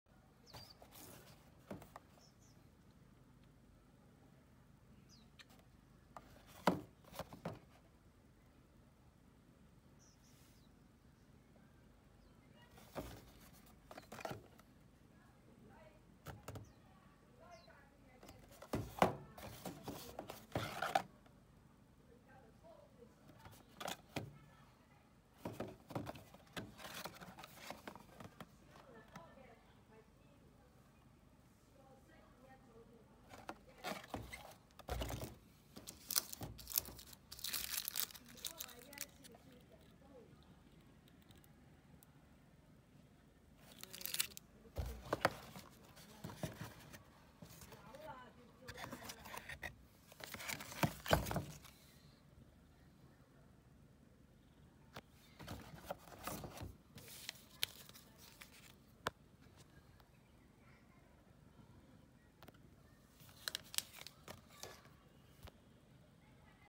{4K60fps}川宁沁香柠檬味红茶（袋泡调味茶)25包包装. Twinings Lemon Scented Tea 25 pack packaging.
斯里兰卡红茶融入柠檬芬芳，清香沁心。